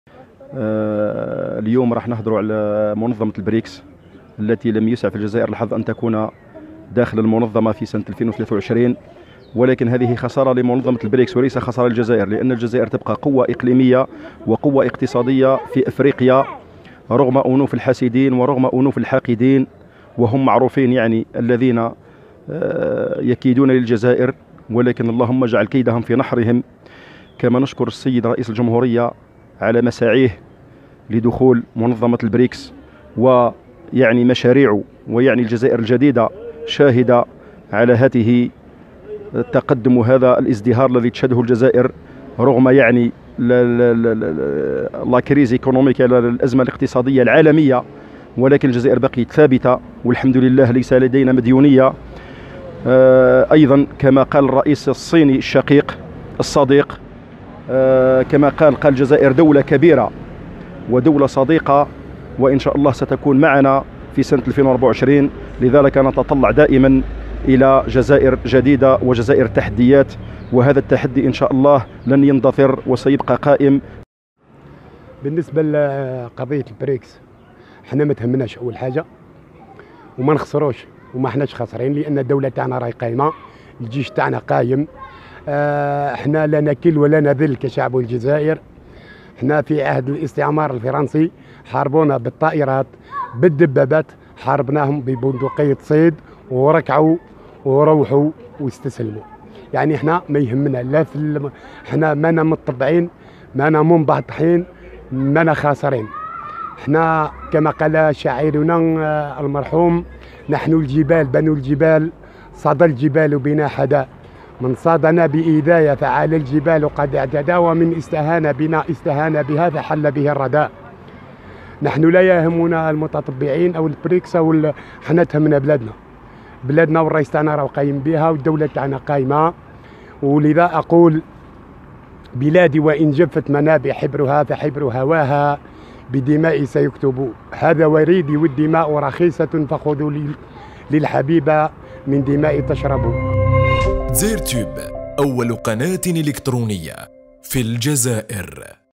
شاهد مواطنوا خنشلة يقولون كلام قوي عن الجزائر بلادنا قوية اقليميا و اقتصاديا وكل خير فيها
شاهد.. مواطنوا خنشلة يقولون كلام قوي عن الجزائر..بلادنا  قوية اقليميا و اقتصاديا وكل خير فيها  والبريكس هو الخاسر الأكبر وليس الجزائر